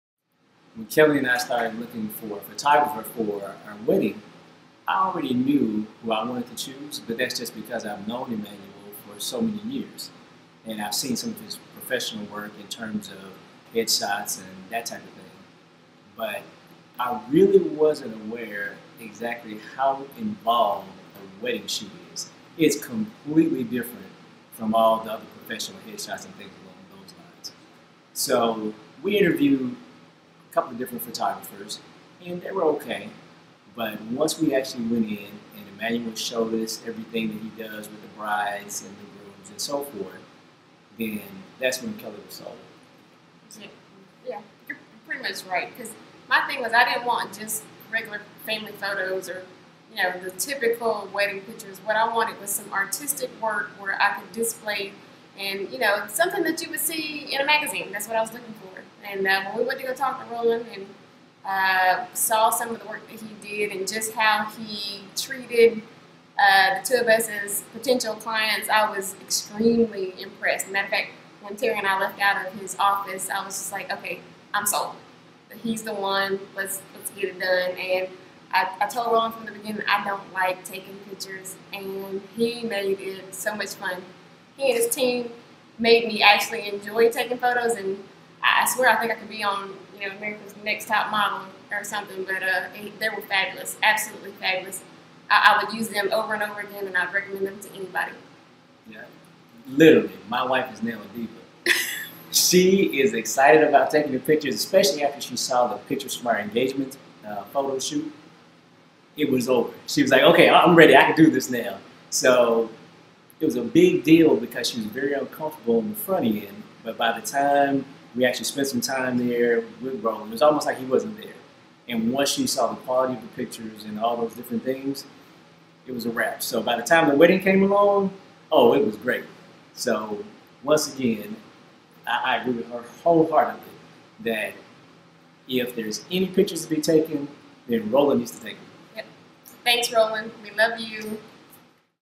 Kellee & Terry Wedding OutDoor Reshoot Interview m4v
One of our Awesome wedding couples Kellee and Terry Stone talk about their experience with ROLAND'S Photography. Wedding July 6, 2013